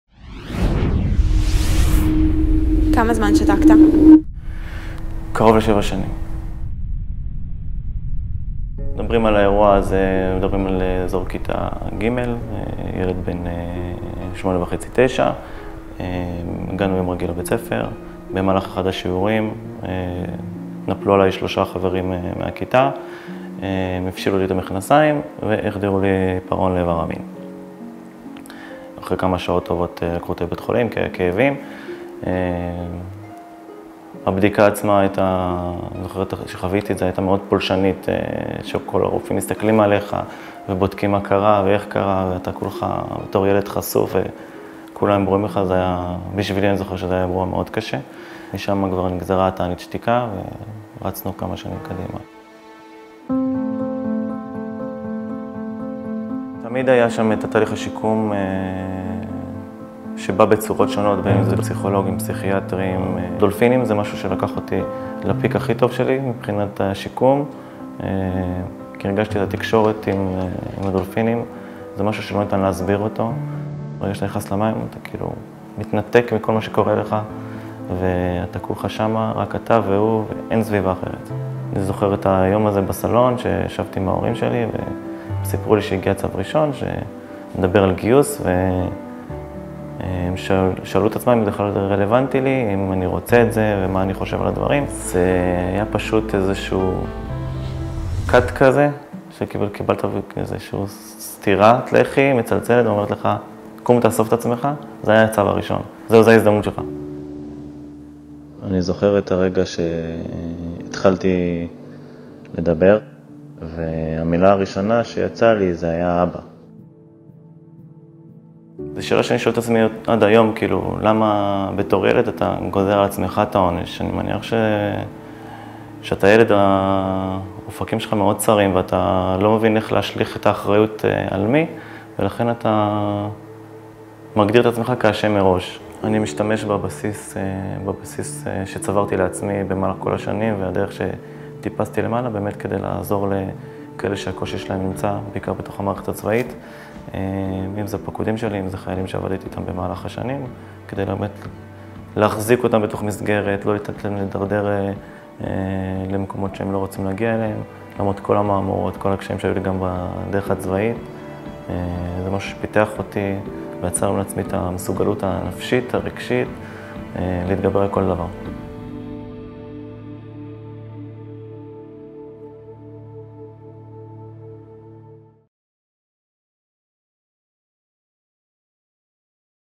הילד שלא דיבר עד שמכתב אחד גרם לו לשבור את השתיקה | צה״ל
במשך 7 שנים ארוכות לא הוציא רס"ר ז'אק סוויסה מילה מהפה בעקבות התעללות מינית שעבר בגיל 9 - עד שמכתב אחד גרם לו לשבור את השתיקה.